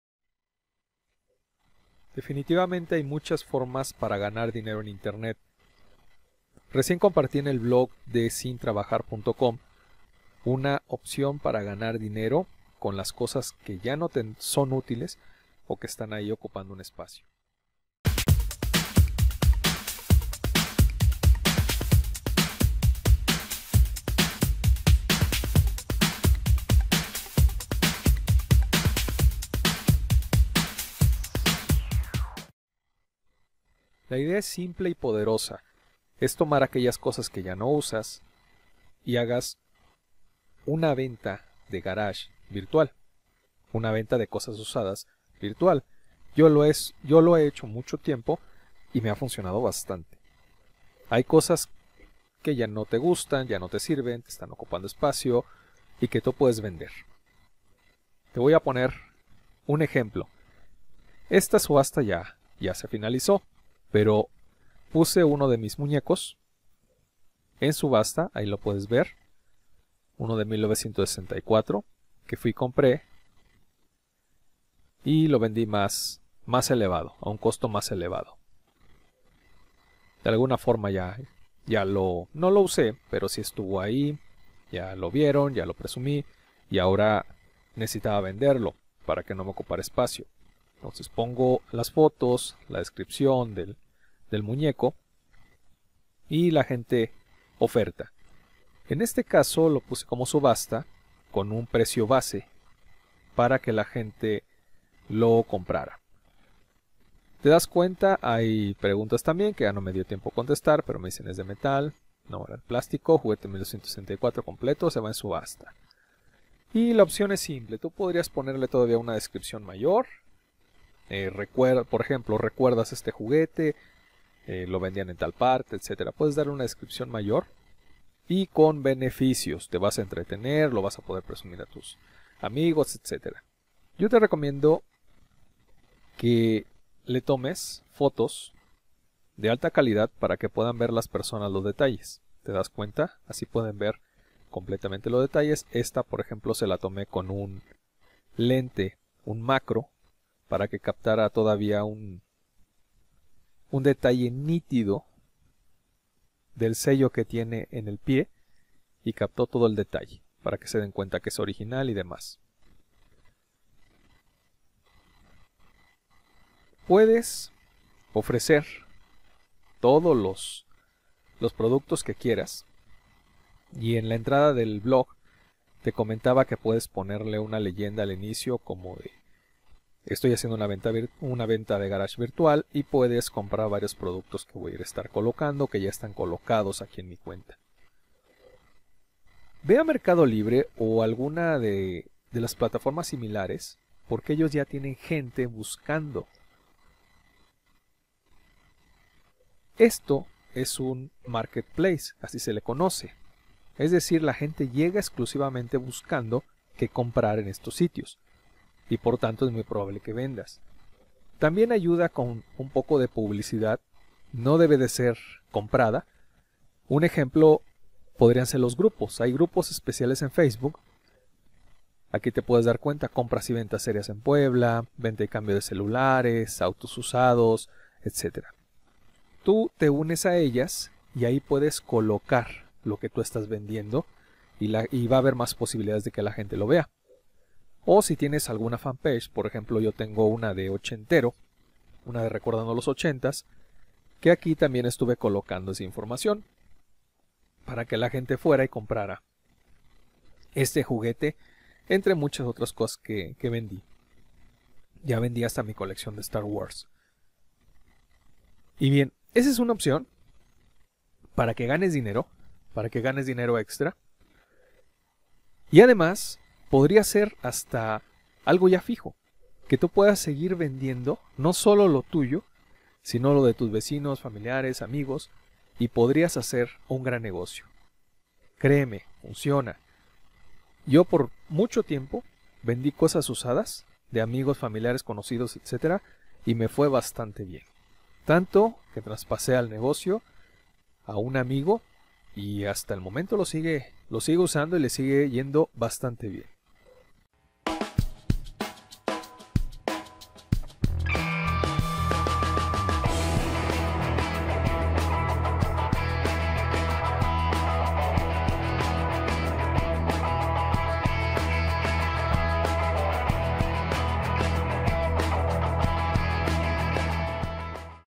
Cómo ganar dinero con una venta de garage virtual | ganar dinero sin invertir y desde casa
Únete a la comunidad para aprender cómo ganar dinero en internet

Cómo ganar dinero con una venta de garage virtual. Te explico cómo ganar dinero sin invertir, con negocios en internet. Esta estrada del blog te ayudará a aprender aún más